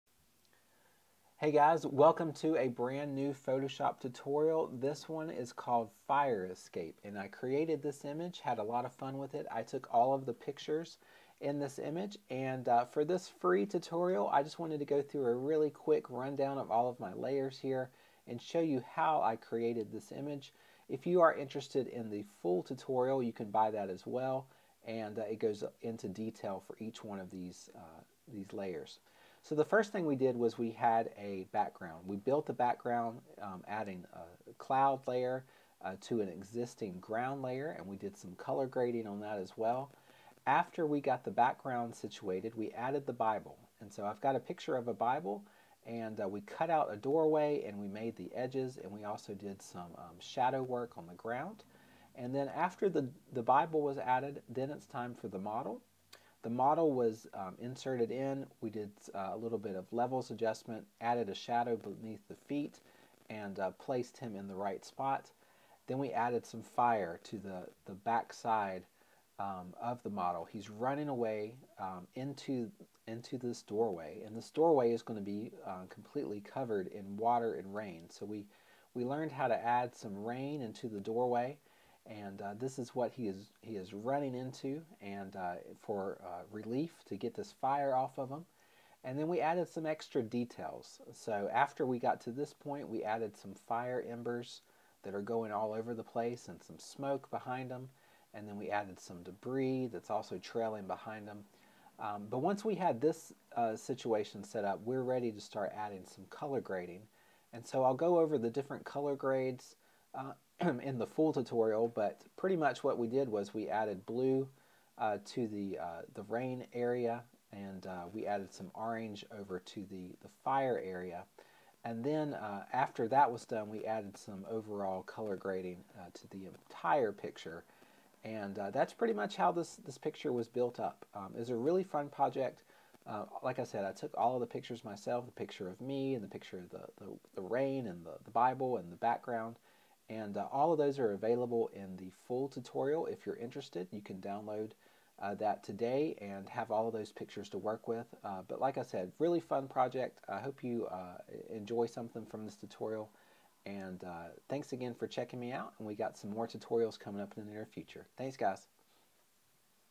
Photoshop Tutorial - Fire Escape
This was a fun project that I created recently, using all pictures that I had taken myself. I have included all of the stock images I used for this image so you can practice on your own. The video is about 75 minutes long, and I go through step by step how I created this image, from the first sketch to the final color grading. Hope you enjoy!

Step 1: Rough Sketch + Stocks
Step 2: Assemble the Background
Step 3: Adding the Bible
Step 4: Adding the Model
Step 5: Adding the Flames
Step 6: Adding the Raindrops
Step 7: Adding in Details
Step 8: Final Polishing
Step 9: Bonus Edits